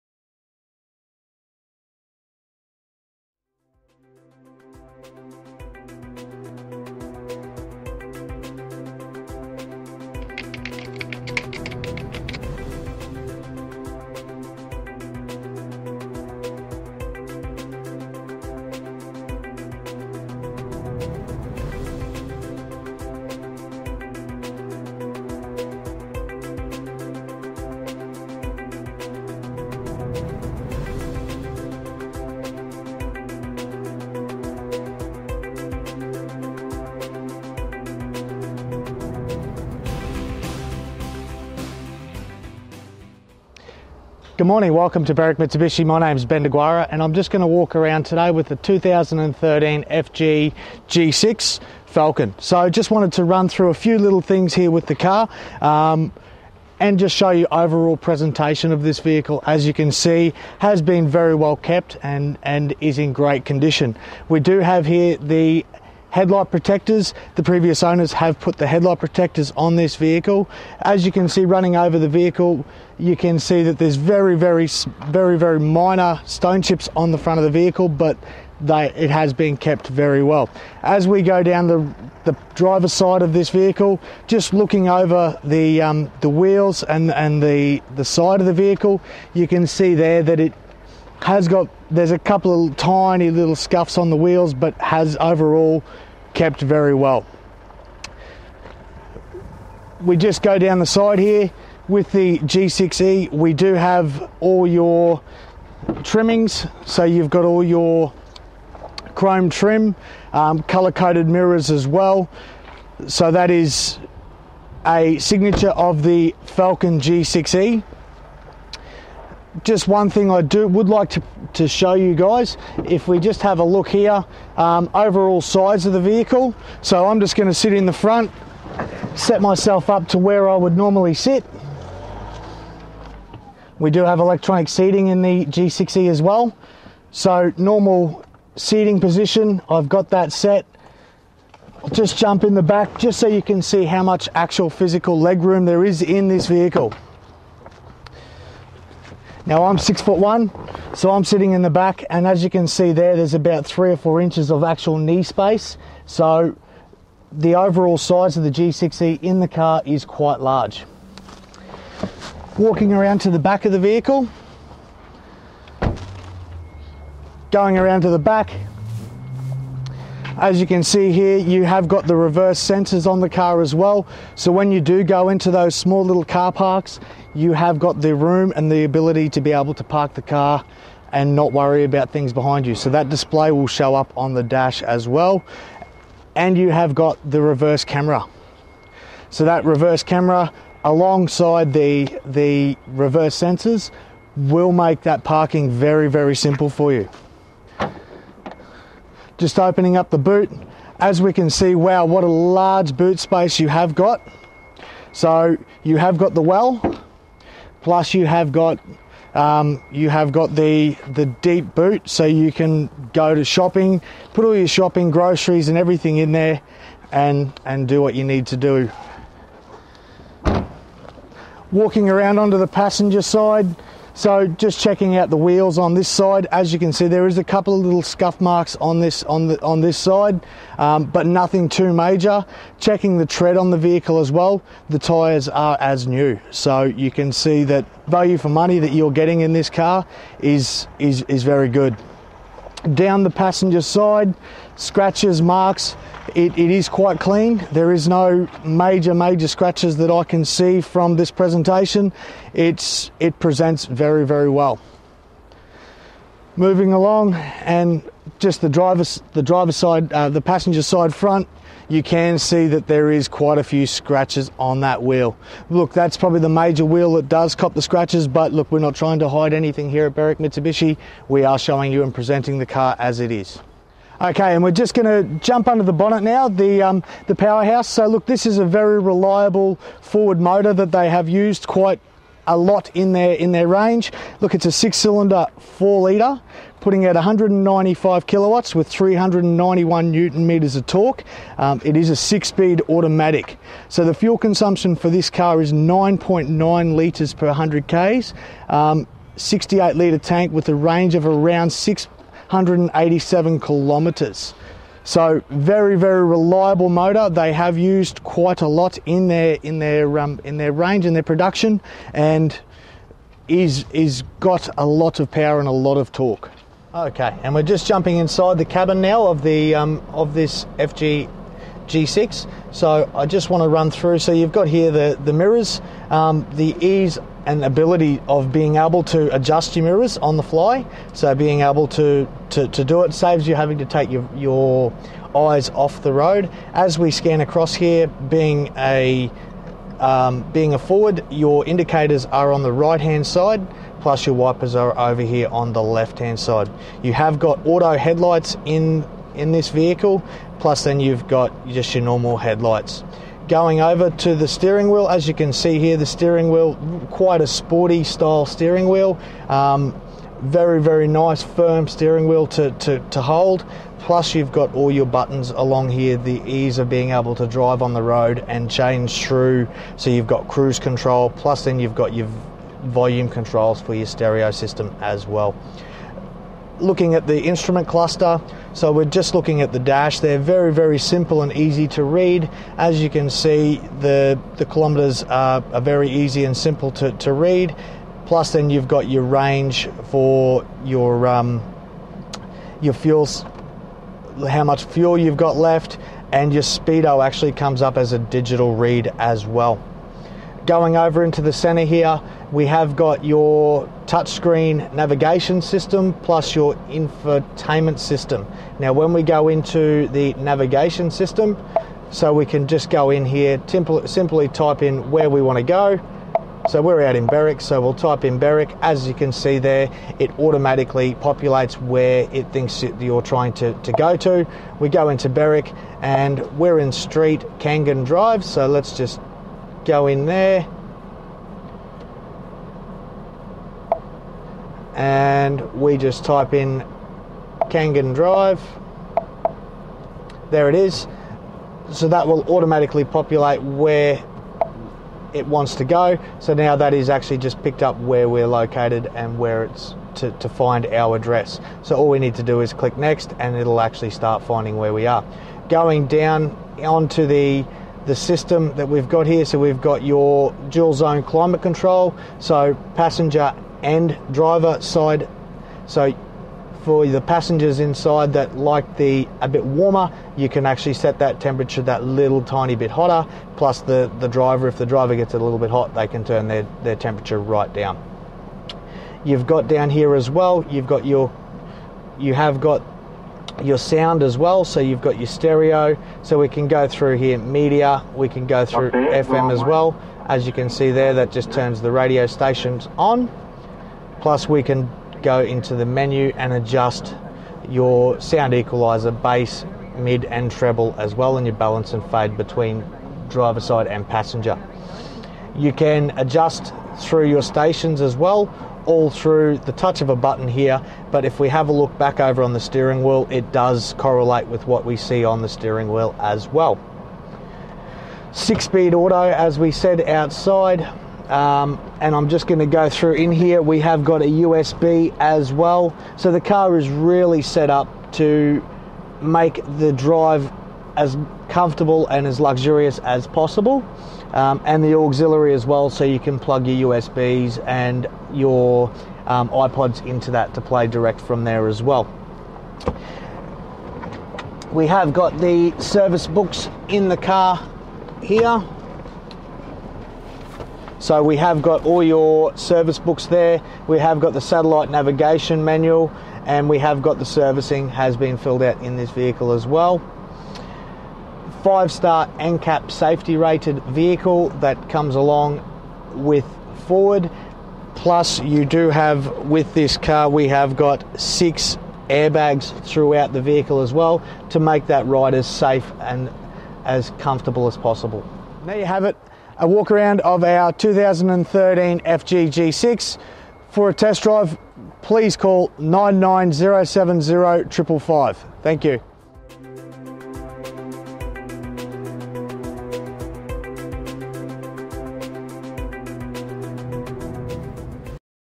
B5981 - 2013 Ford Falcon G6E FG MkII Auto Walkaround Video
A video walkaround tour of the B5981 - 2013 Ford Falcon G6E FG MkII Presented by Ben Deguara Used Car Sales at Berwick Mitsubishi,